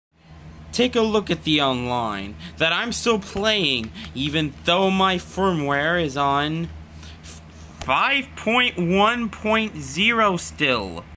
To people who hasn't updated their switch yet...
Don't do it if you wanna stay online multi-player for free.(if it comes at the cost of not going to the eshop.)